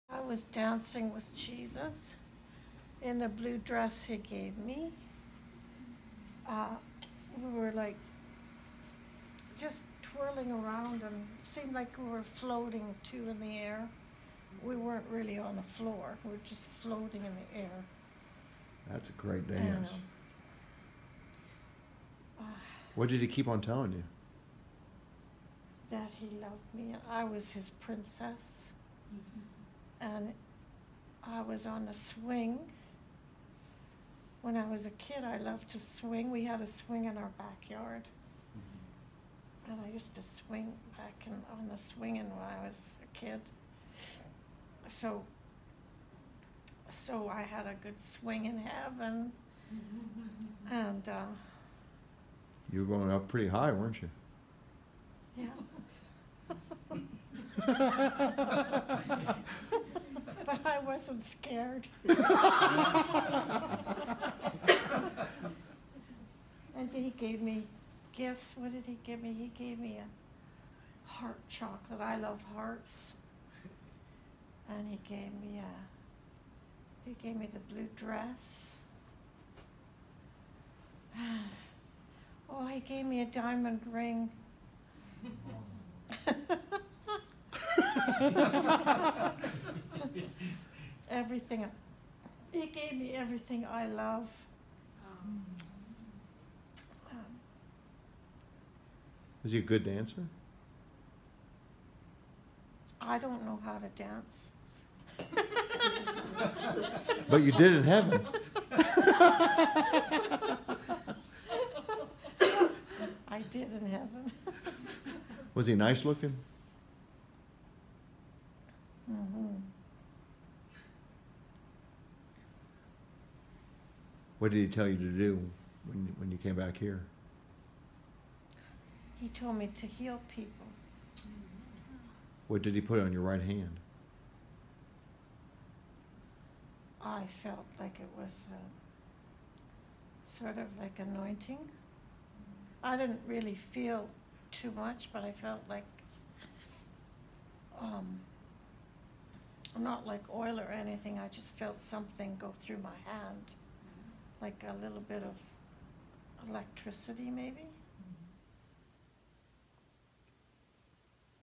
British Columbia: Swinging with Jesus in Heaven
After a British Columbia seminar we ministered to a woman who was caught up into the Third Heaven!